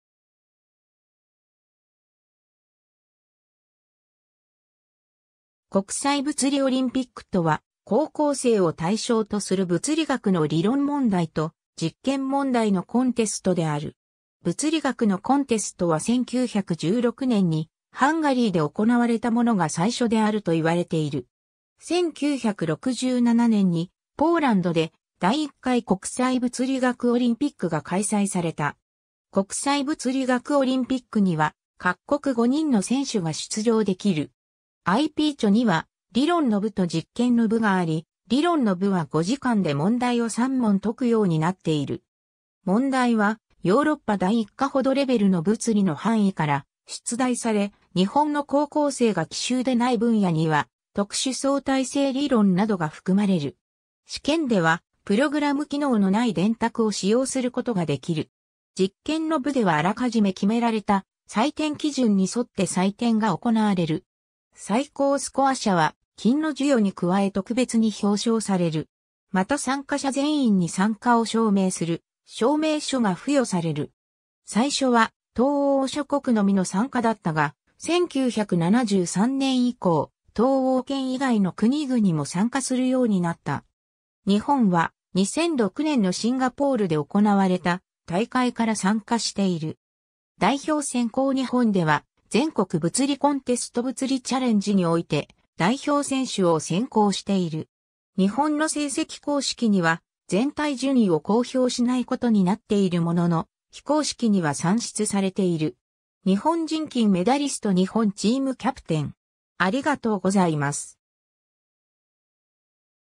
国際物理オリンピック
国際物理オリンピック（こくさいぶつりオリンピック、IPhO : International Physics Olympiad）とは、高校生を対象とする物理学の理論問題と実験問題のコンテストである。
 物理学のコンテストは1916年にハンガリーで行われたものが最初であるといわれている。
1967年にポーランドで第1回国際物理学オリンピックが開催された。
 国際物理学オリンピックには各国5人の選手が出場できる。
IPhOには理論の部と実験の部があり、理論の部は5時間で問題を3問解くようになっている。
問題はヨーロッパ第1課程レベルの物理の範囲から出題され、日本の高校生が既習でない分野には特殊相対性理論などが含まれる。
試験ではプログラム機能のない電卓を使用することができる。
実験の部ではあらかじめ決められた採点規準にそって採点が行われる。
 最高スコア者（Absolute Winner 絶対勝者）は、金の授与に加え特別に表彰される（日本人受賞者は1名）。
また参加者全員に参加を証明する証明書が付与される。
 最初は東欧諸国のみの参加だったが、1973年以降、東欧圏以外の国々も参加するようになった。
日本は、2006年のシンガポールで行われた大会から参加している。
 代表選考 日本では、全国物理コンテスト物理チャレンジにおいて代表選手を選考している。
 日本の成績 公式には全体順位を公表しないことになっているものの、非公式には算出されている。
 日本人金メダリスト 日本チームキャプテン